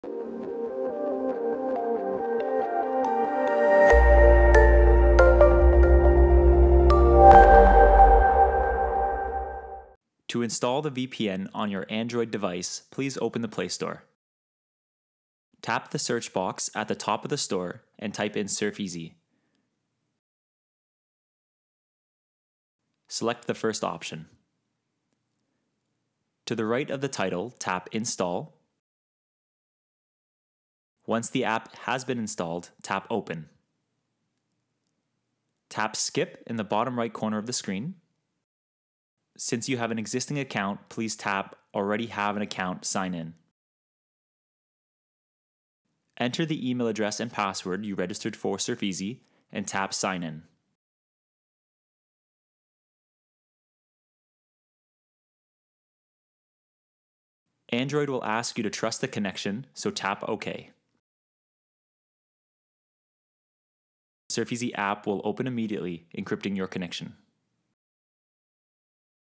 Install SurfEasy VPN on Android
A brief tutorial on how to download and install SurfEasy VPN to protect the privacy and security on your Android mobile or tablet.